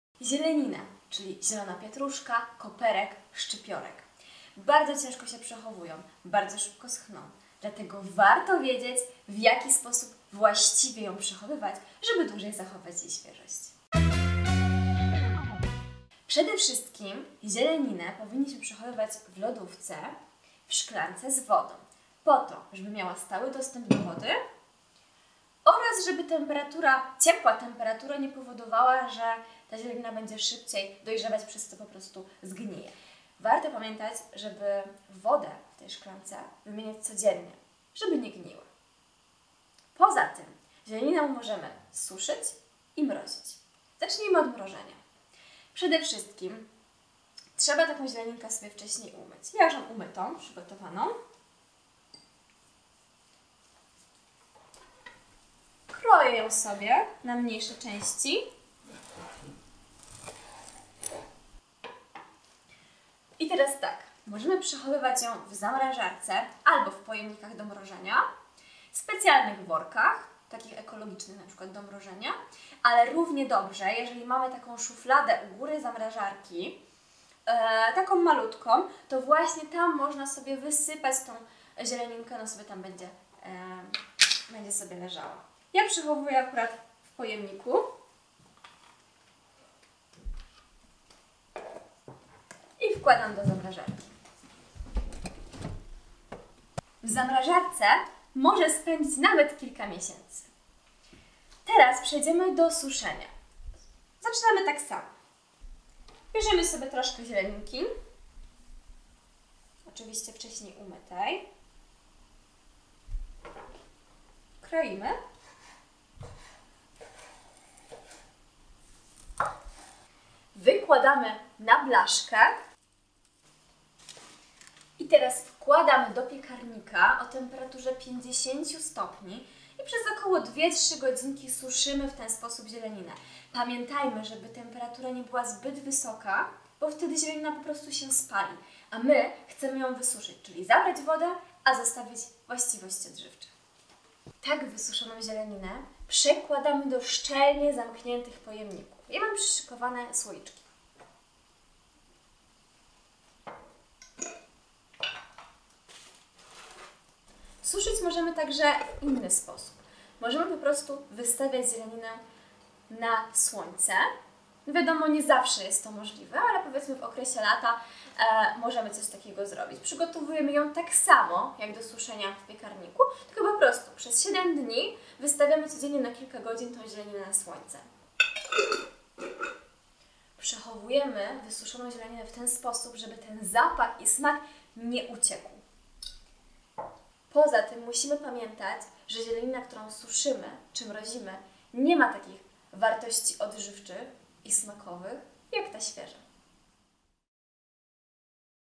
Jak przechowywać żywność: zielenina
Czy Zielenina w twoim domu również szybko traci swoją świeżość? Nie zdążysz się obrócić, a tu już uschnięte gałązki koperku? Mamy na to świetny sposób! "Don’t waste me!" pomoże Ci walczyć z marnowaniem żywności.